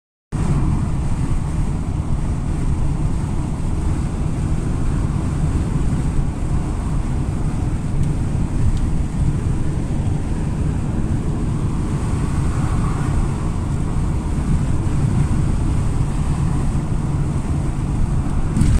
La neve vista da Dalila
Breve video del paesaggio che stiamo attraversando by Dalila il 13/12/2019